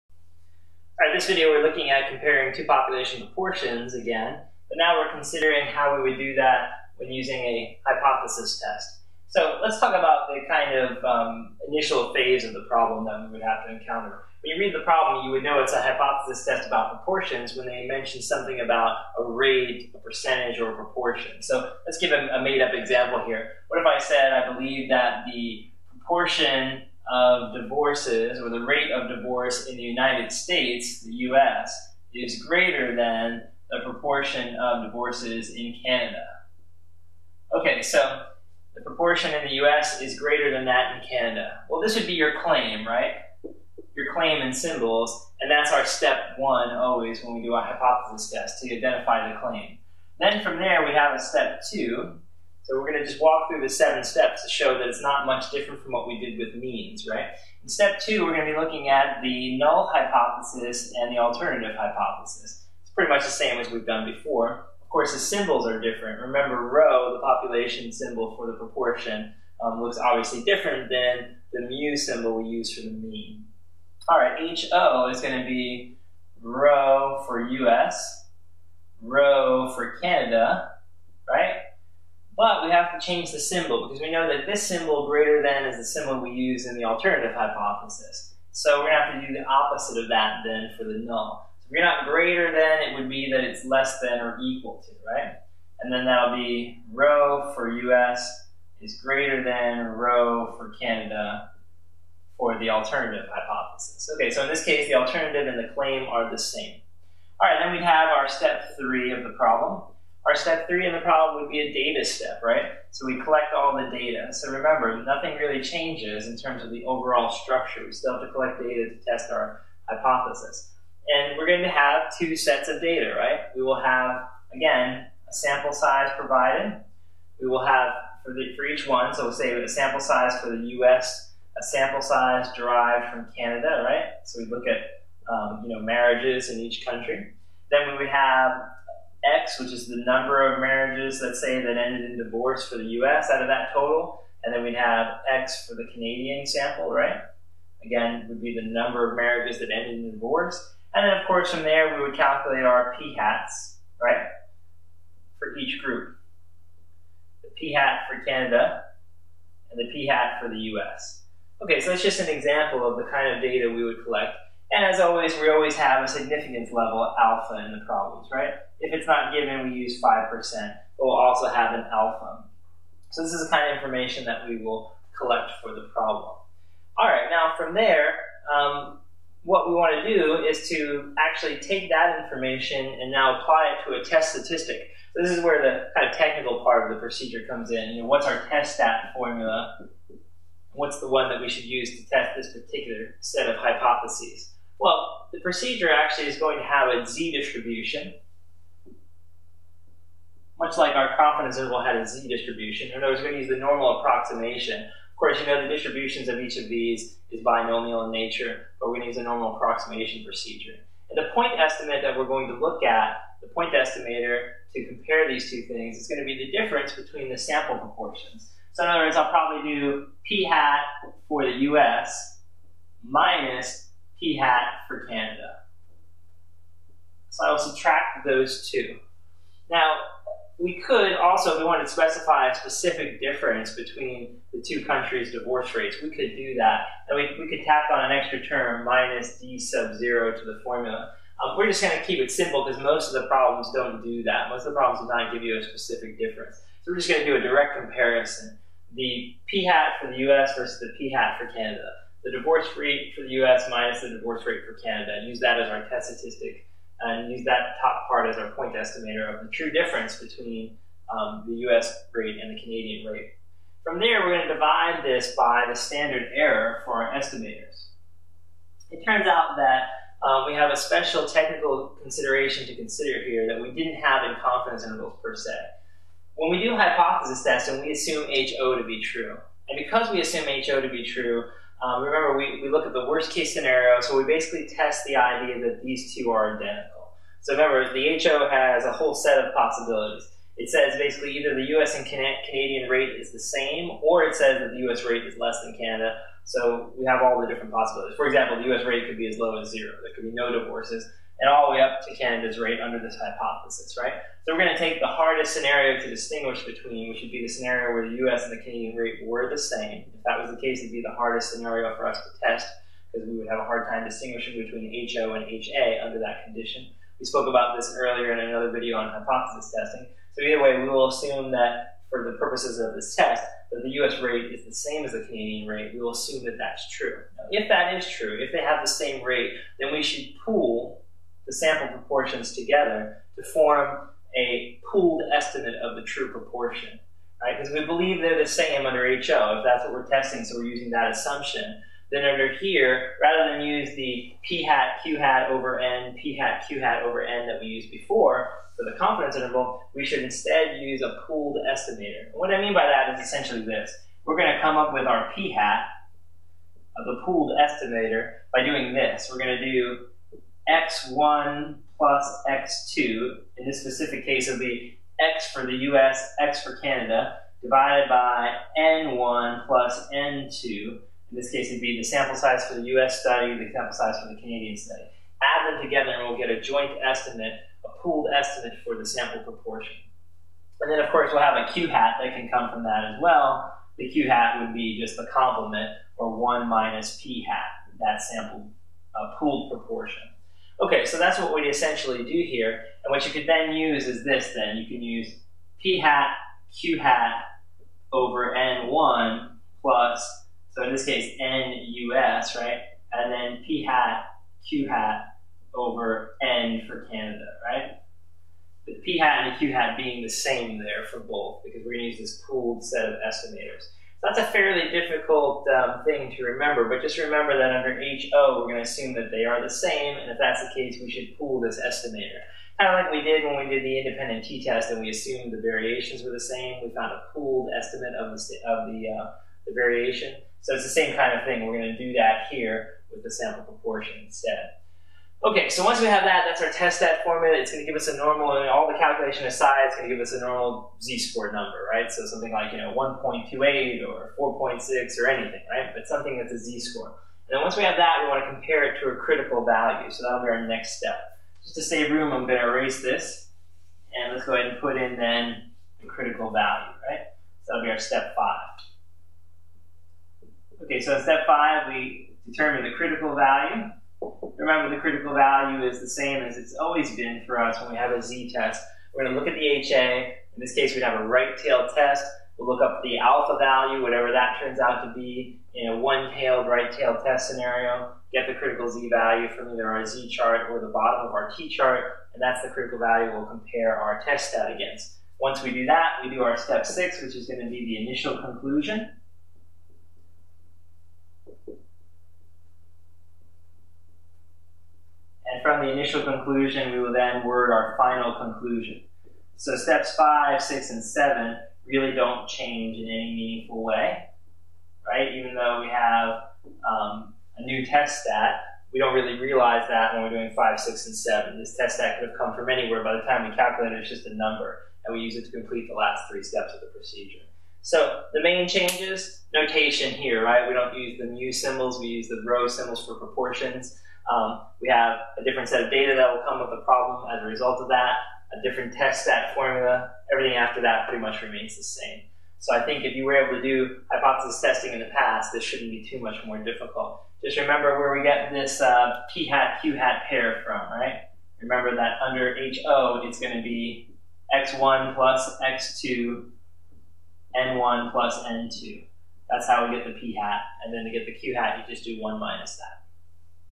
Testing the Difference between Two Population Proportions
In this video, we discuss the method for testing hypothesis about the true difference between two population proportions.